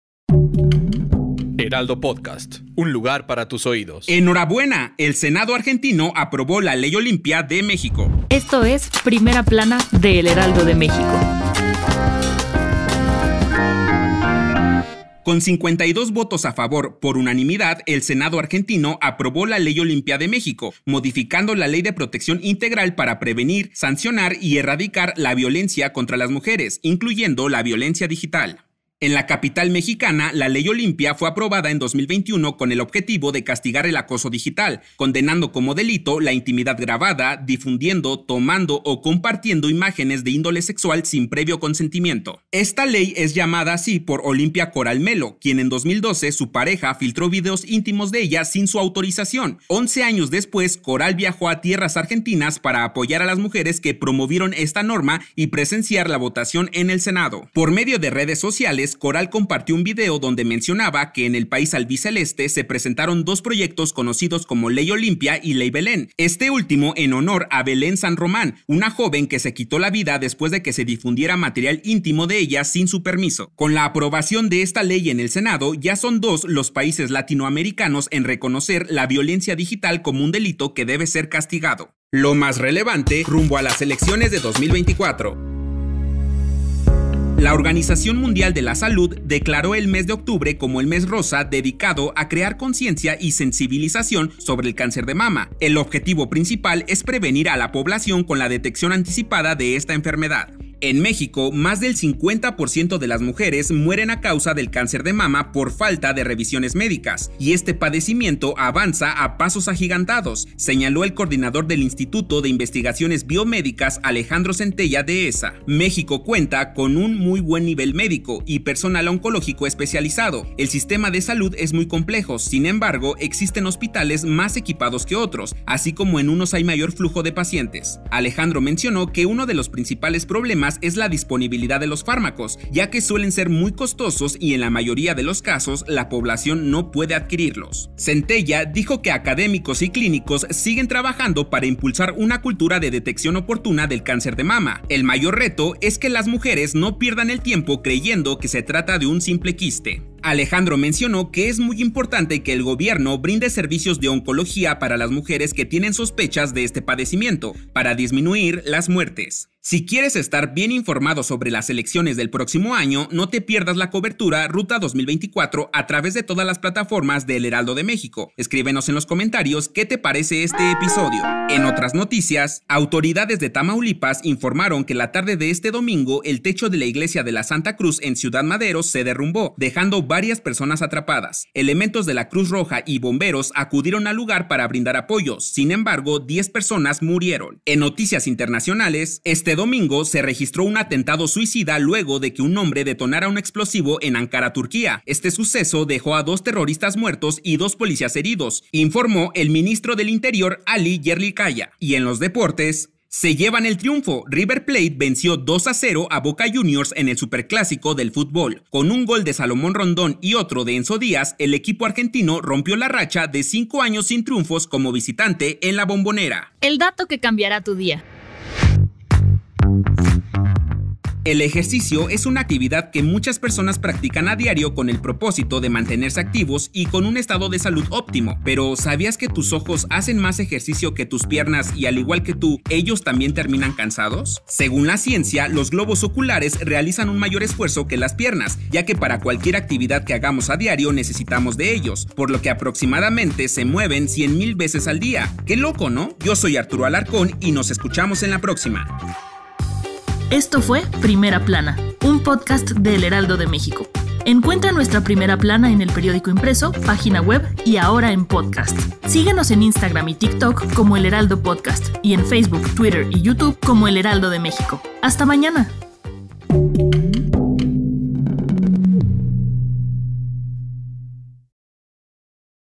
Ley Olimpia llega a Argentina | Primera Plana
La Ley Olimpia mexicana llegó a Argentina, ya que el Senado de ese país aprobó con 52 votos, por unanimidad, la modificación a la Ley de Protección Integral para Prevenir, Sancionar y Erradicar la Violencia contra las Mujeres, incorporando la violencia digital.

En México, la Ley Olimpia que castiga el acoso digital y tipifica como delito la intimidad sexual grabada, tomar, difundir imágenes o mensajes de contenido sexual sin previo consentimiento, fue aprobada desde 2018 y hasta 2021 en diferentes estados.